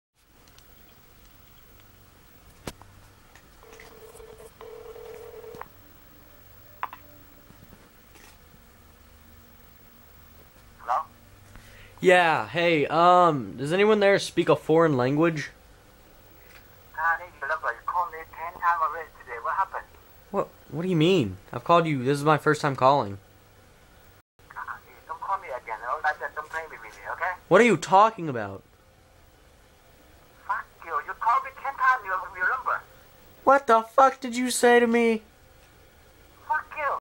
Chinese food prank call
Chinese dude gets mad at me